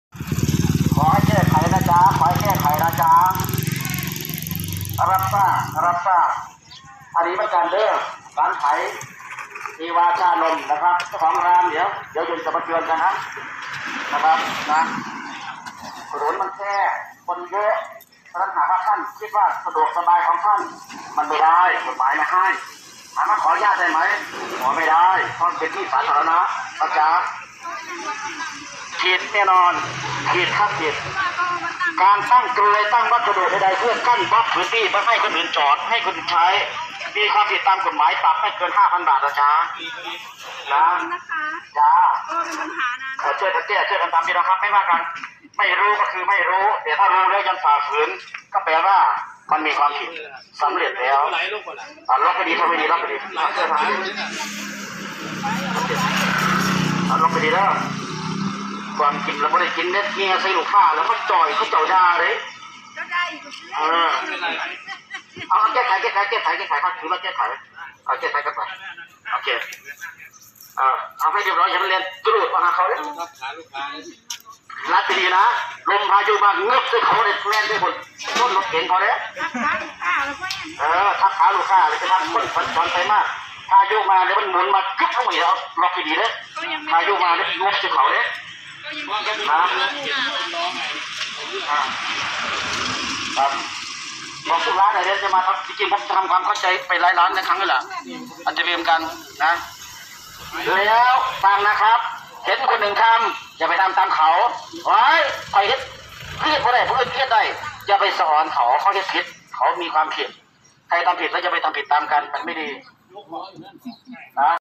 ปัญหาทุกอย่างมีทางแก้ ถ้อยทีถ้อยอาศัยกัน เทศกิจจัดระเบียบร้านค้า
ปัญหาทุกอย่างมีทางแก้ ถ้อยทีถ้อยอาศัยกัน